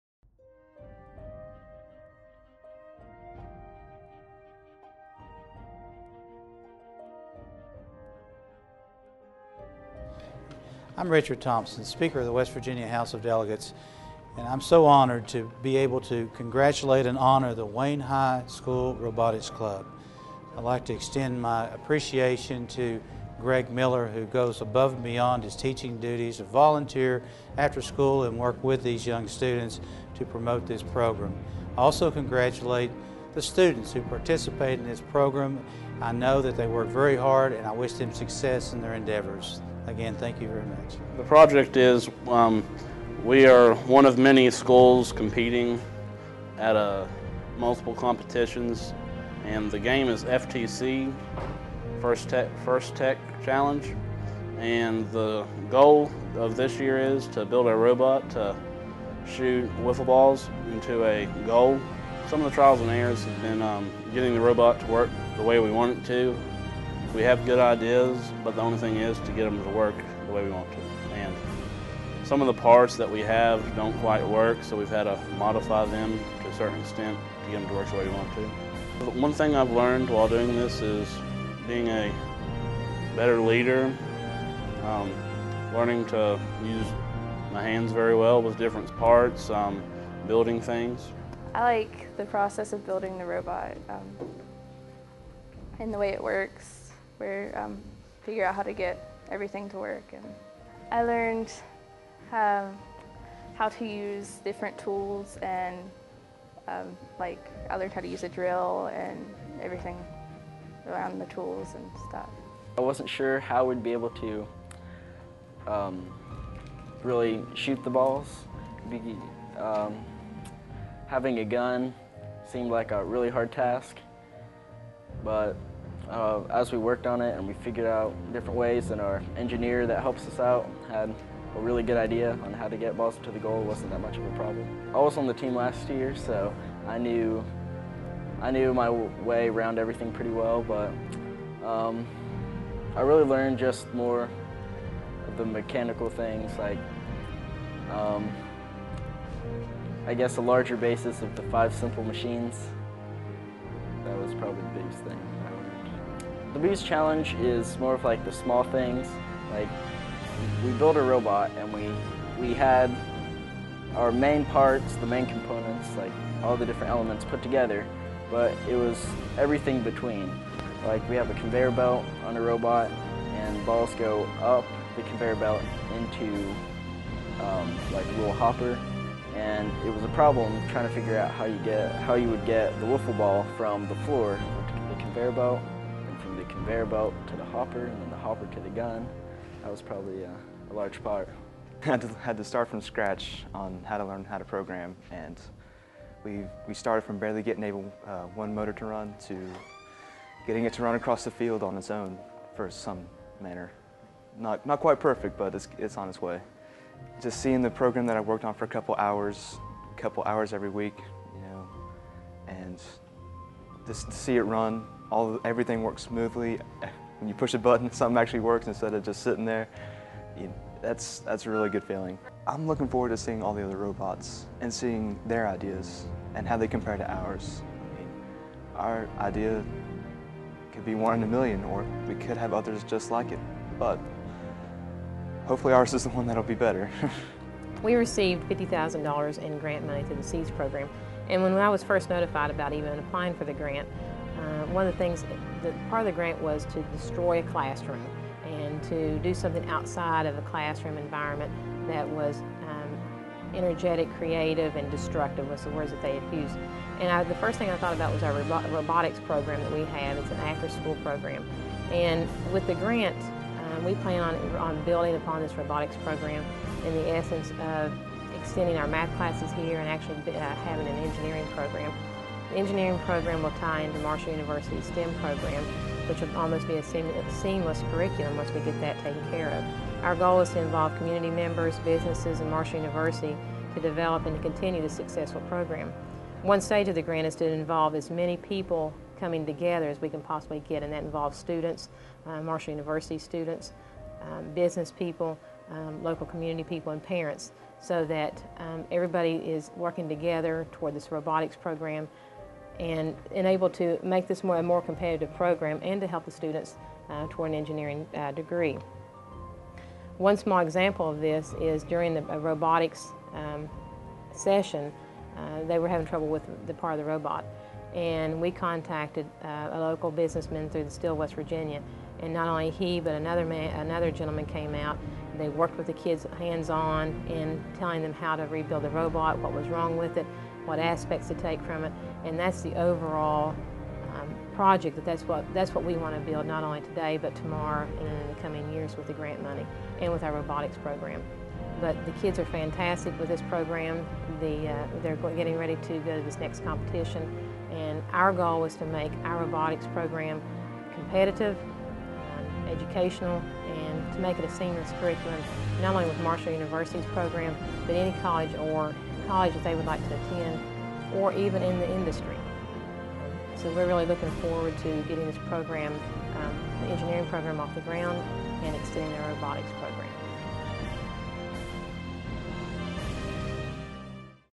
Wayne High School Robotics Team 3273
This is a look at the Wayne high School Robotics team.
Team 3273 would like to mention the following sponsors who helped make this possible:  American Electric Power, Richard Thompson-Speaker of the West Virginia House of Delegates and Bill Thompson who served as the Engineering Mentor.